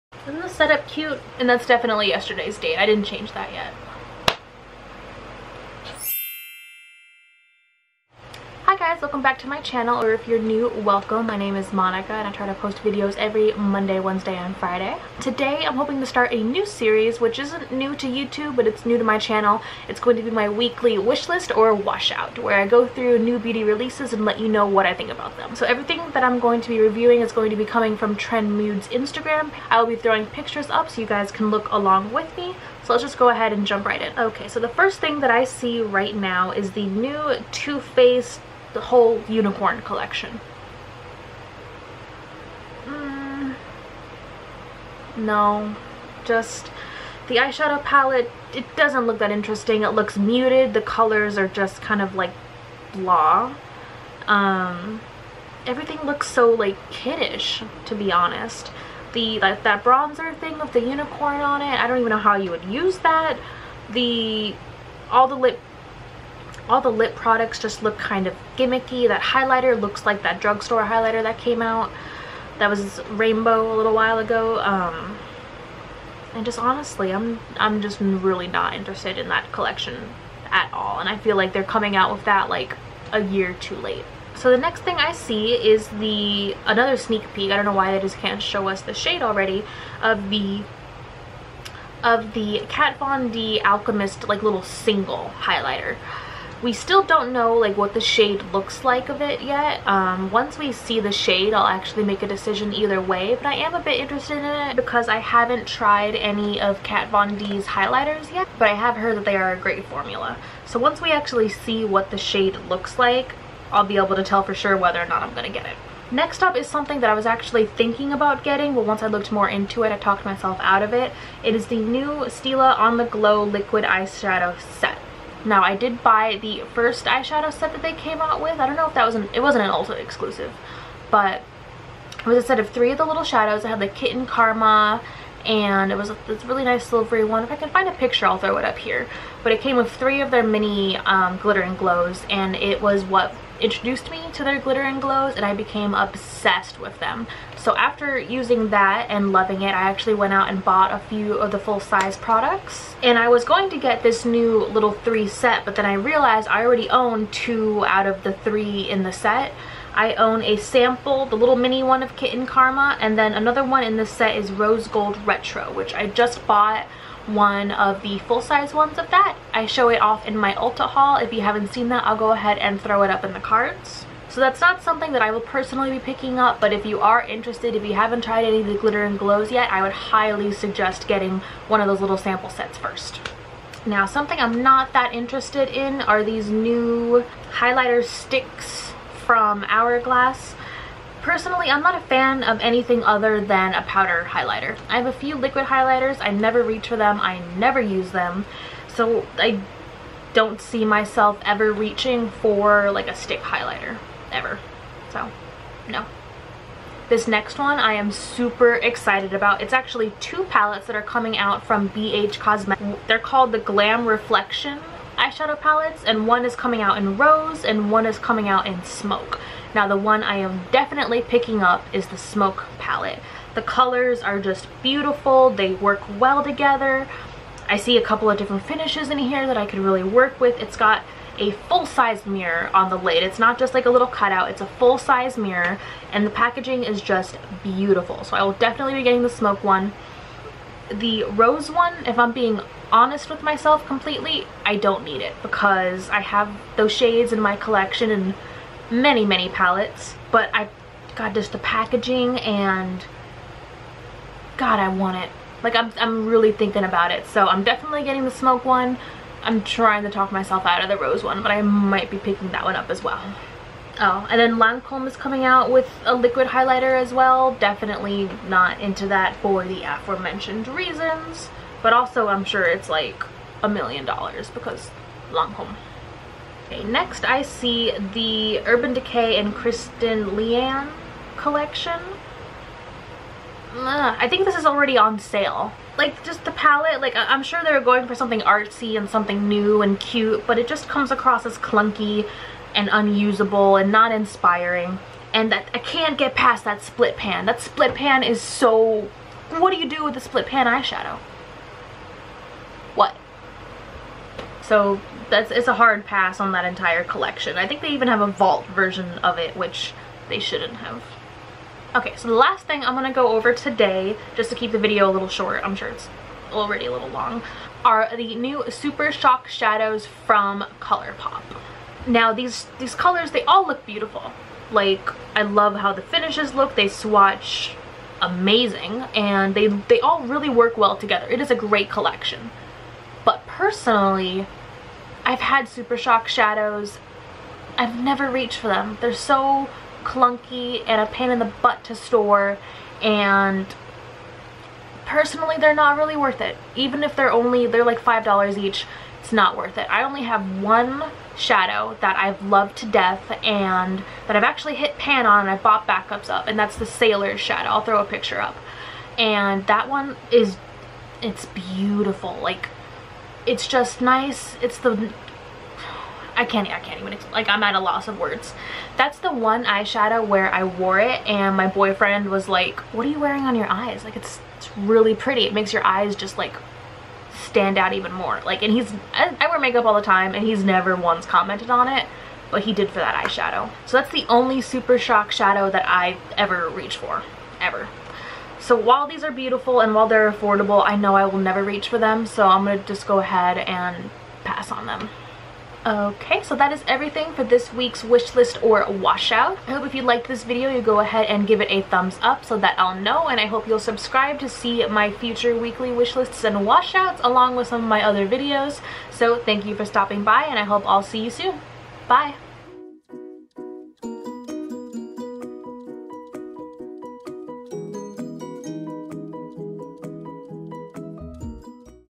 Weekly Wishlist or Washout!? | NEW Makeup Releases
Hi everyone! Welcome to a new series I am hoping to do weekly, my Weekly Wishlist or Washouts! All pictures were taken from Trendmood's Instagram